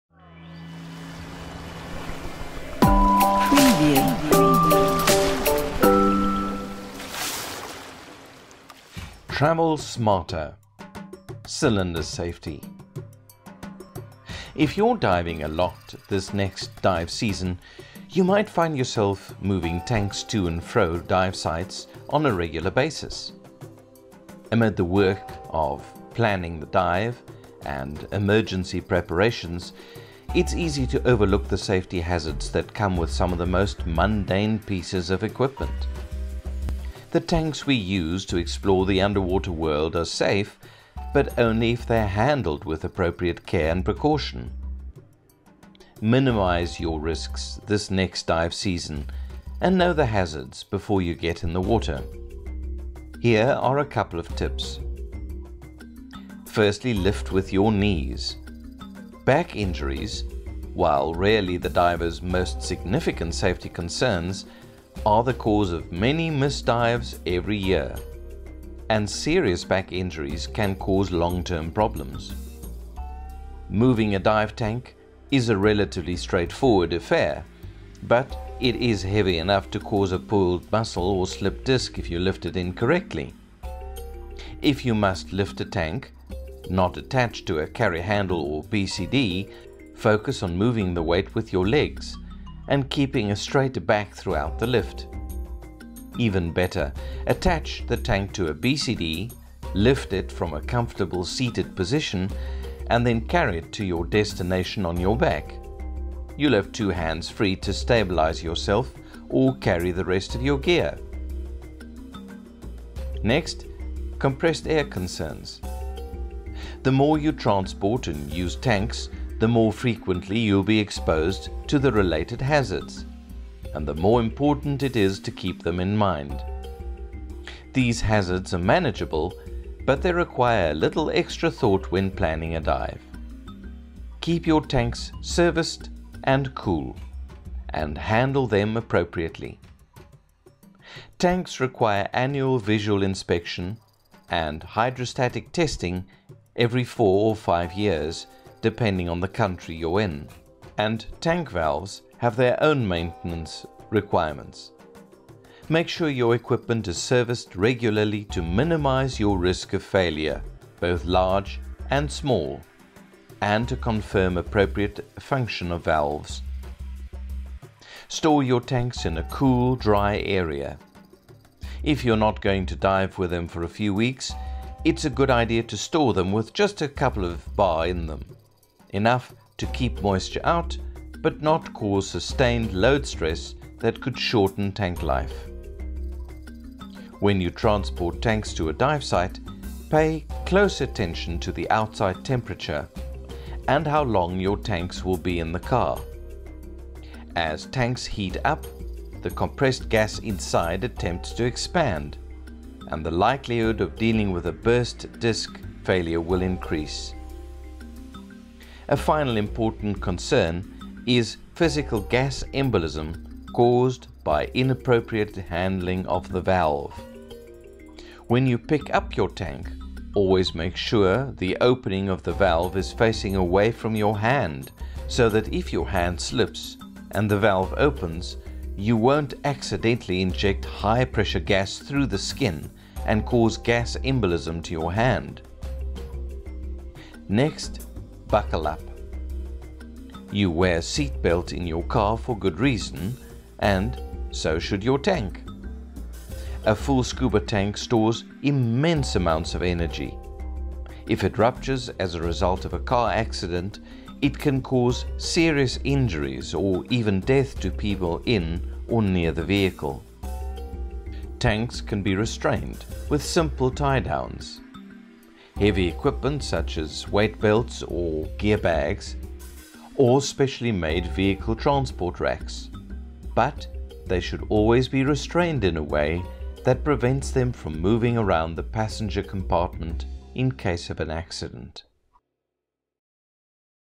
Travel Smarter: Cylinder Safety
If you're diving a lot this summer you might find yourself moving tanks to and from dive sites on a regular basis. Amid the work of dive planning and emergency preparations, it's easy to overlook the safety hazards that come with some of the most mundane pieces of equipment. The tanks we use to explore the underwater world are safe, but only if they're handled with appropriate care and precaution. Minimize your risks this summer, and know the hazards before you get in the water.